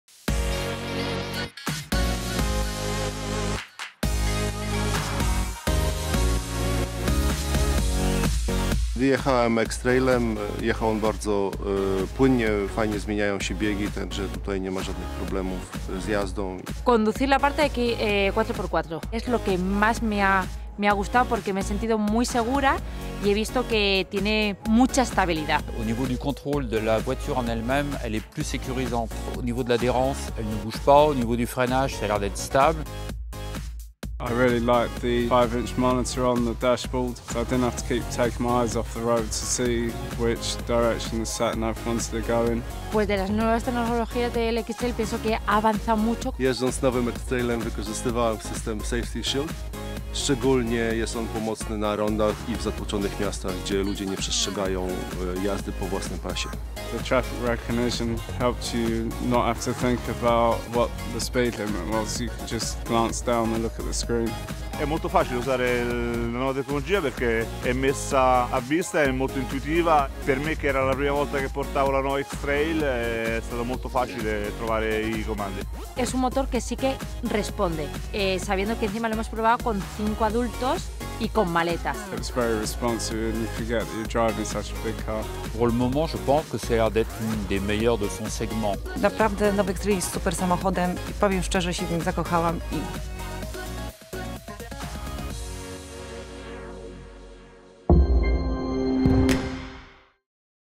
Nissan X-TRAIL - saugos sistemos ir dinaminės savybės
Išgirskite pirkėjų nuomonę apie naujojo Nissan X-TRAIL saugos sistemas ir dinamines savybes.